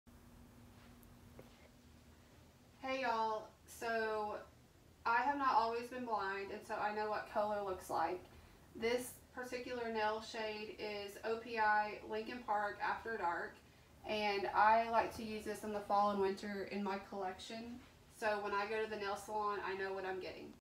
💅 How does a Blind Girl choose a nail color?
Outfit details
My dark brown hair is in a half up half down style
I am wearing black, faux, leather wide  leg pants
White ribbed tank top
Medium wash, chambray shirt with ruffle detail on the   shoulders, left un buttoned
Leopard print golden goose sneakers with Black star
My jewelry is pearl, hoop, earrings, mixed metal bracelet with pearl detail, my Apple Watch with leopard print, watch band, and my wedding ring
Nail color is OPI Lincoln park after dark, which is a dark, dark purple color that almost looks black

In this video, I am sitting in a brown leather chair, showing the camera, my fresh gel manicure, and explaining how I know what nail color I am getting at the nail salon.